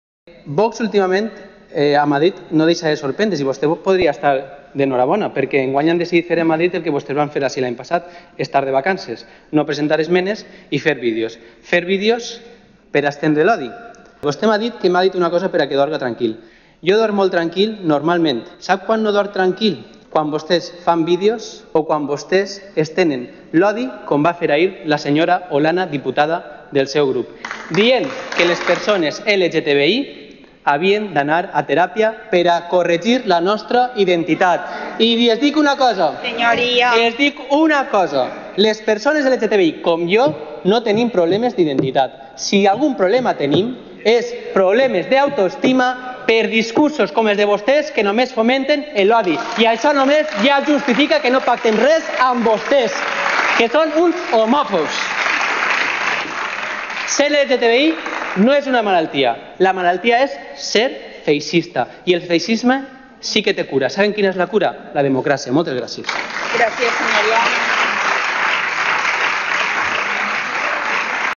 🌈"Ser LGTBI no és una malaltia; ser feixista, sí" - Fran Ferri, contra l'homofòbia de V🤮x"
Discurs del Síndic-Portaveu de Compromís a les Corts, Fran Ferri.